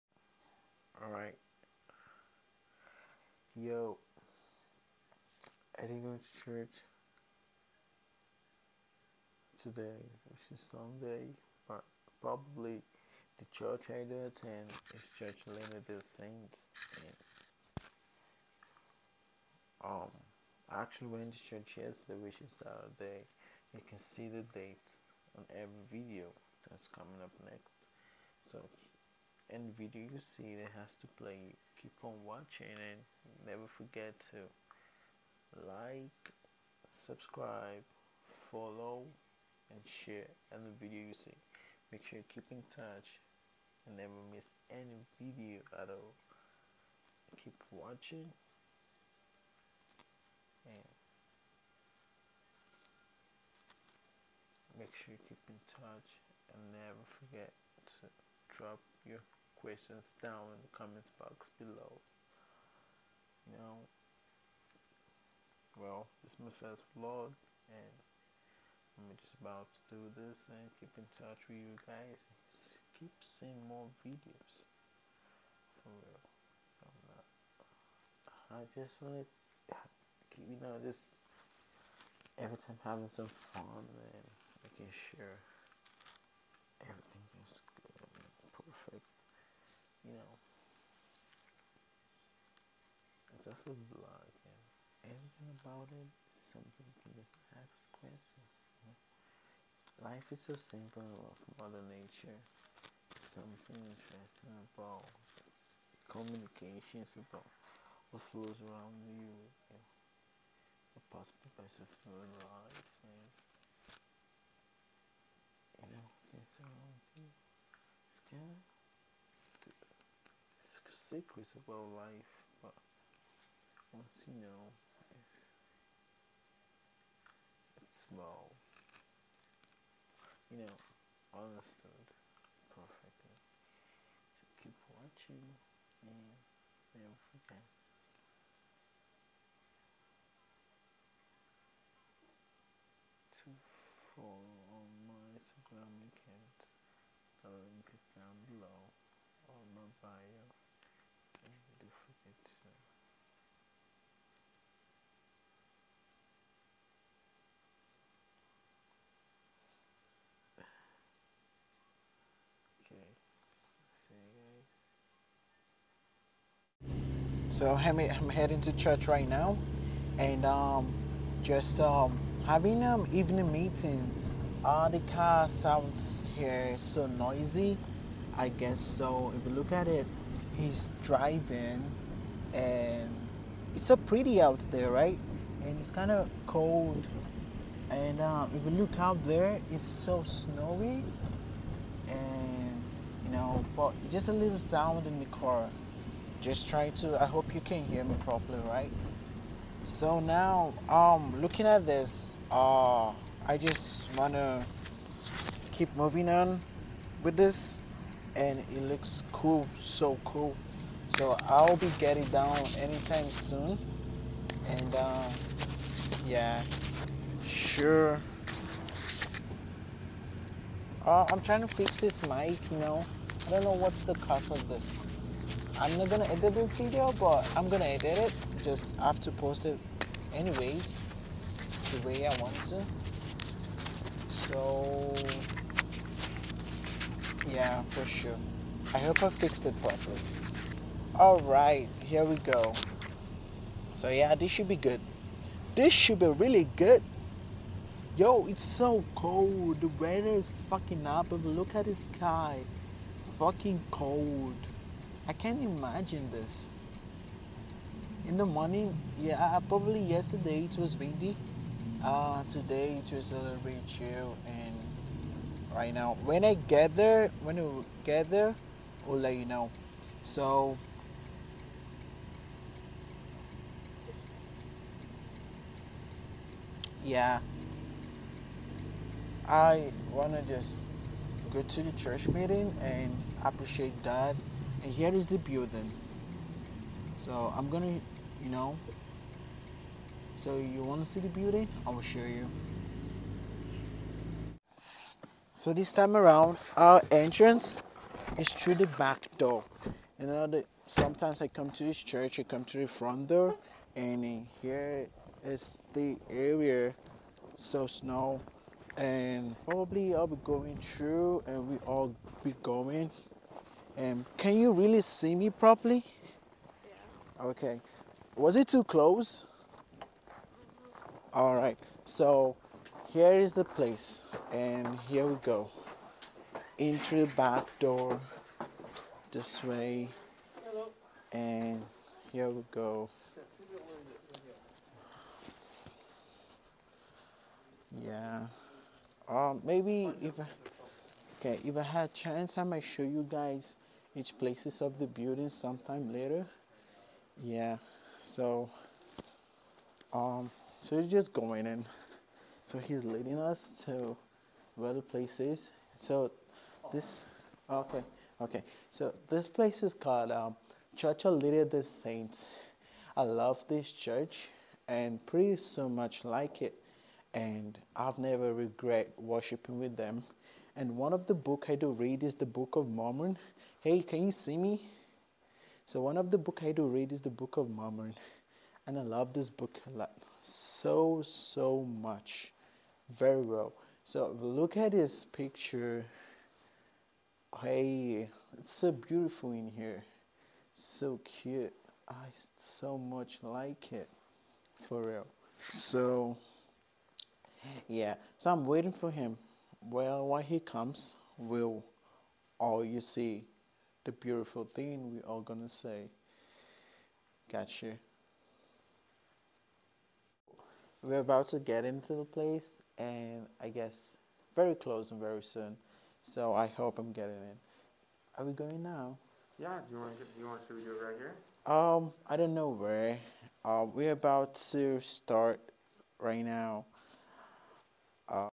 christian johnson - First Vlog / Social Religious
Few descriptions about my Church

Recently, just started my first Vlog.